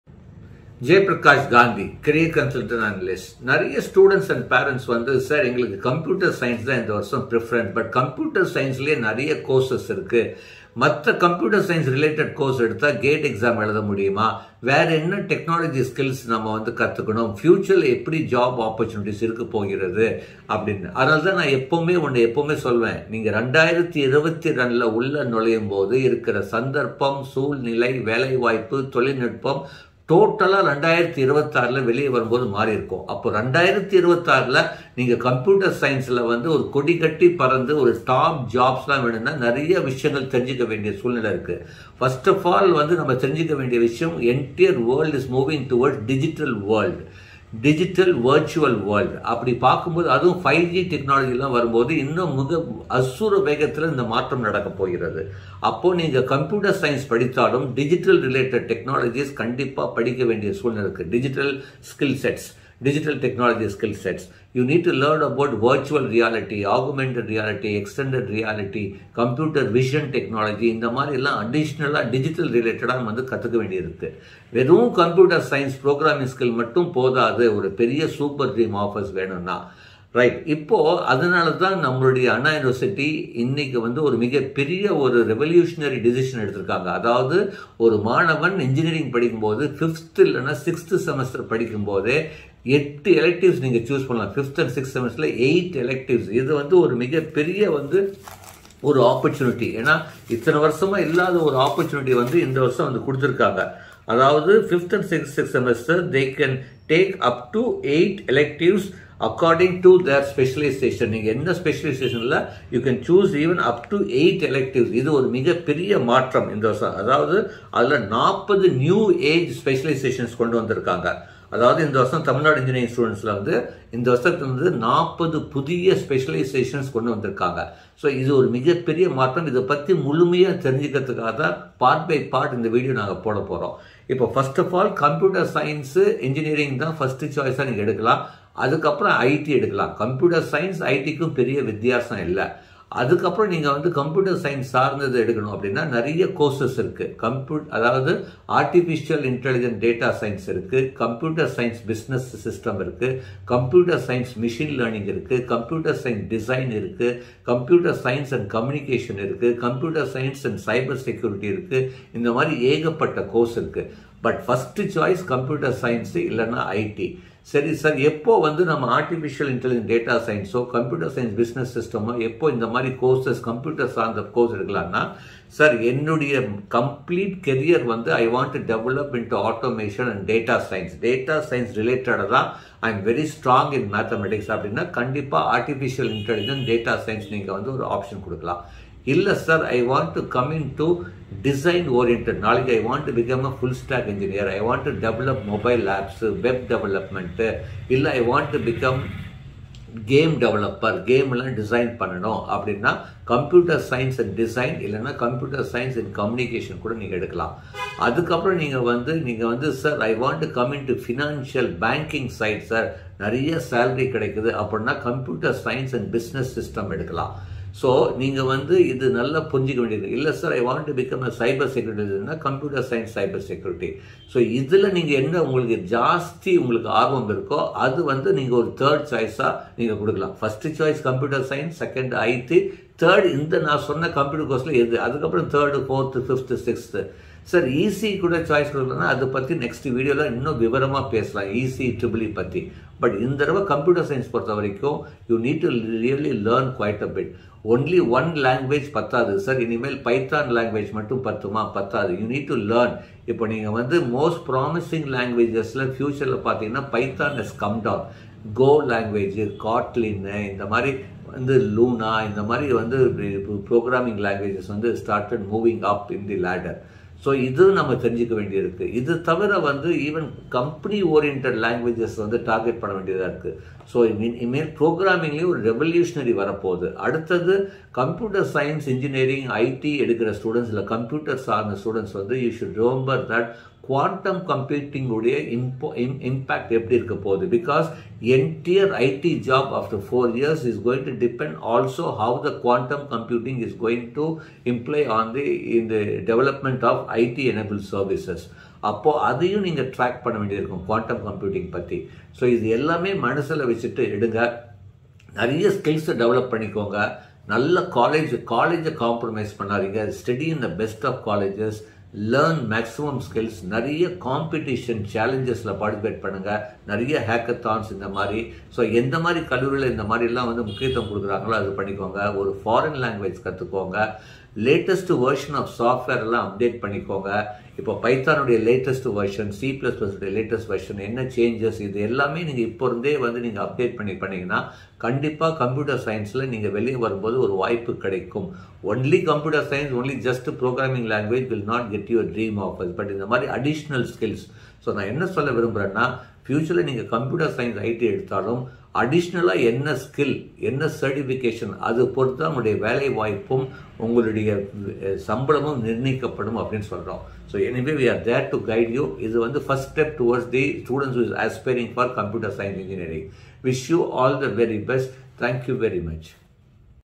Engineering Courses: Which is better? CSE vs IT vs AI vs ML vs CSBS vs DS | Career Guidance
To all the students and parents who are confused on which computer related branch/ courses they should take - this video is for you! As you know there are multiple computer related branches, how do you choose? Which is more important? Watch this video and clarify all your doubts!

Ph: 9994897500